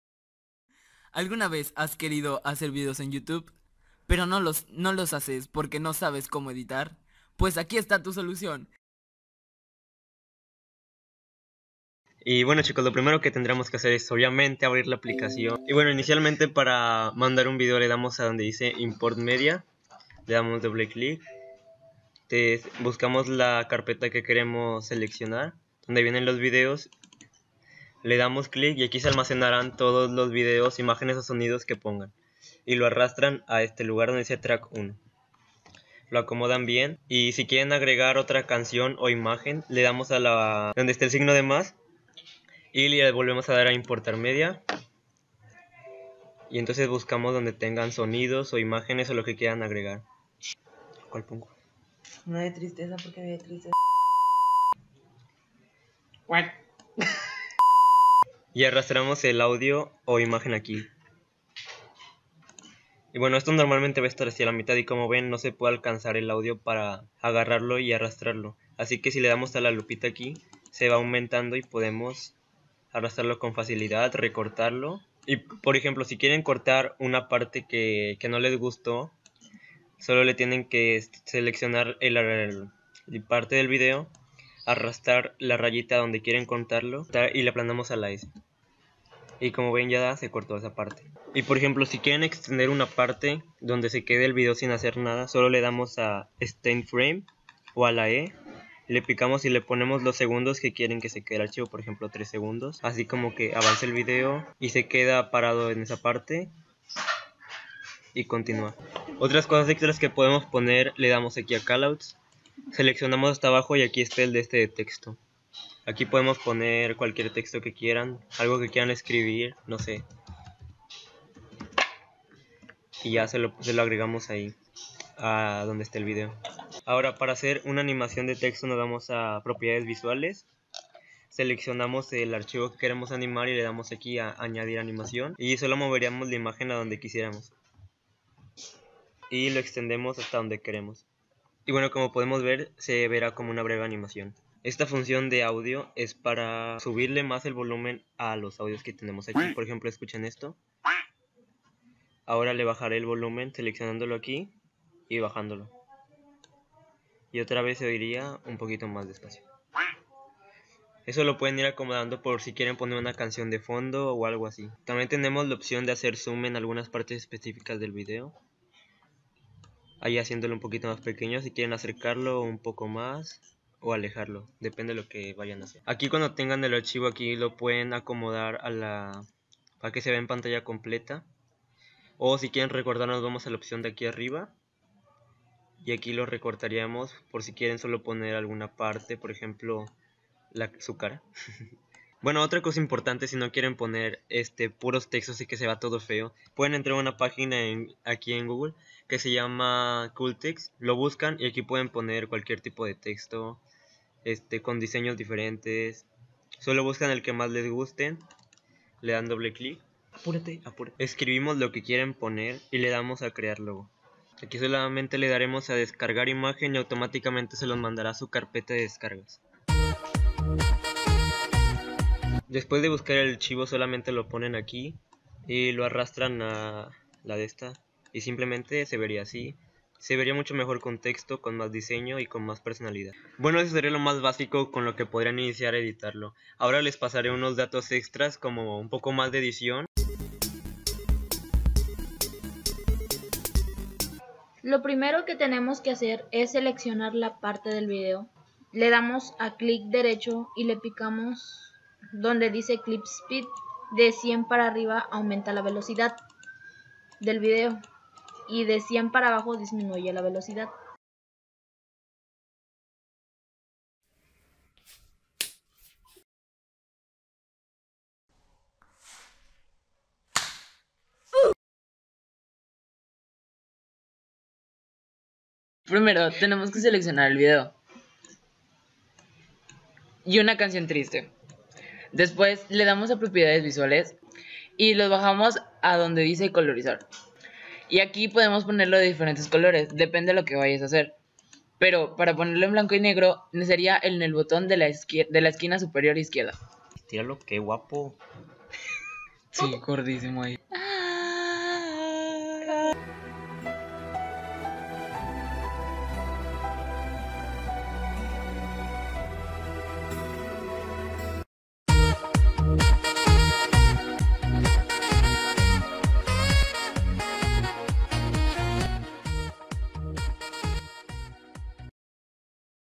¡COMO EDITAR TUS VIDIOS¡
2000 años despues porfin subo un video xD
Pero bueno en este video hare un tutorial de como editar tus feos videos xd.
Y como pueden ver me ayudaron dos amigos que no son youtubers pero me quisieron ayudar.